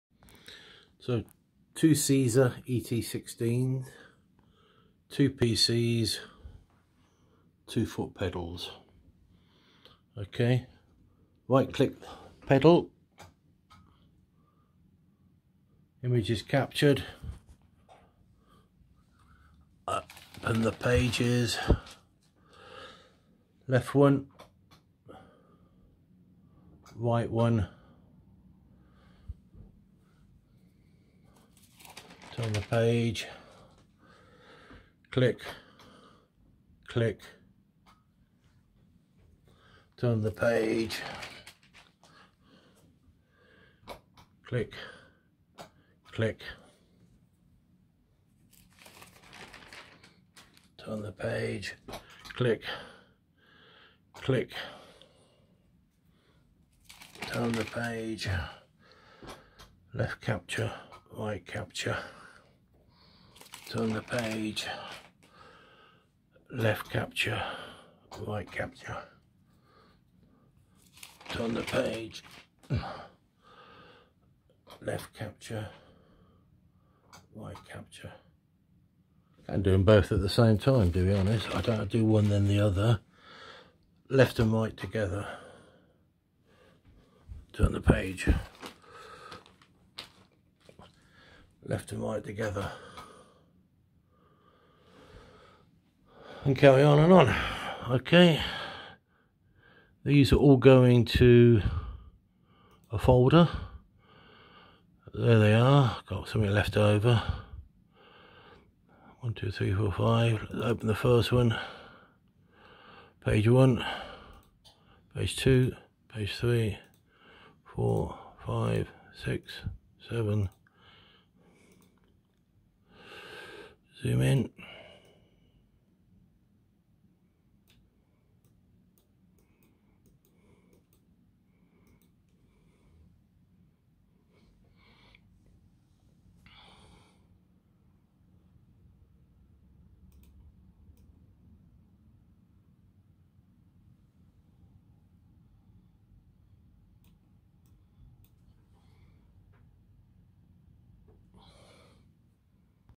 Dual Czur ET Scanners Scanning a Newspaper
Using two Czur scanners to scan left and right pages of a newspaper with incremental file naming and foot pedal switches into a shared folder.  Pages are rotated at the point of capture and use autosizing.  Right camera capture pages 1,3,5,7...etc and left camera 2,4,6,8 etc.